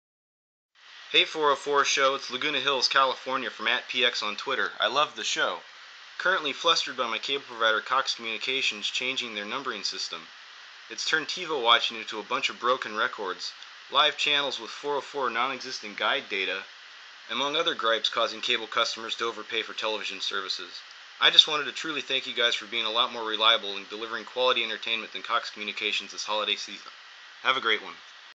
@the404, video voicemail
Subject: 404 video contest entry!

Audio Transcript:

Hey 404!
==  show recordings==
It's Laguna Hills, California from @px on Twitter.
==  flip to me==
 I love the show.
== flip to TV==

Currently flustered by my cable provider, Cox Communications, changing their numbering system.

== flip to TV-guide==
It's turned TiVo watching into a bunch of broken records, live channels with 404, non-existent, guide data, among other gripes causing cableCARD customers to over pay for television services.

I just wanted to truly thank you guys for being a lot more reliable in deliverying quality entertainment than the Cox Communications this holiday season.